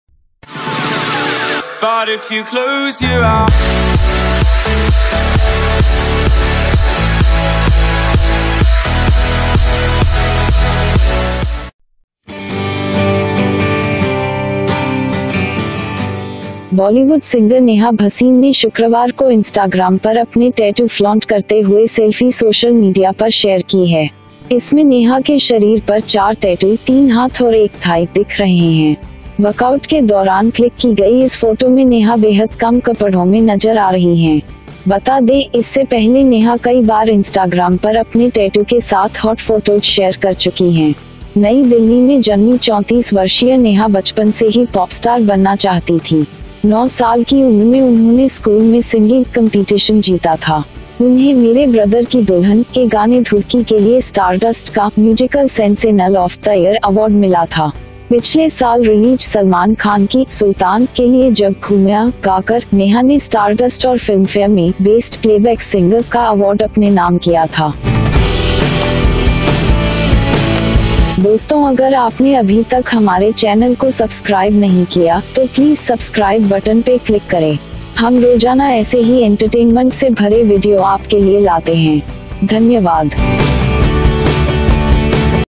सेल्फी में टैटू शो करती दिखी बॉलीवुड की ये सिंगर, देखें हॉट Photos
साल 2017 के फिल्मफेयर अवॉर्ड्स में बेस्ट फीमेल प्लेबैक सिंगर का अवॉर्ड जीतने वाली नेहा ने अपना टैटू फ्लॉन्ट करते हुए सेल्फी सोशल मीडिया पर शेयर की है। बॉलीवुड सिंगर नेहा ने ये फोटो शुक्रवार को इंस्टाग्राम पर अपलोड की है। इसमें नेहा के शरीर पर चार टैटू दिख रहे हैं। वर्कआउट के दौरान क्लिक की गई इस फोटो में नेहा बेहद कम कपड़ों में नजर आ रही हैं।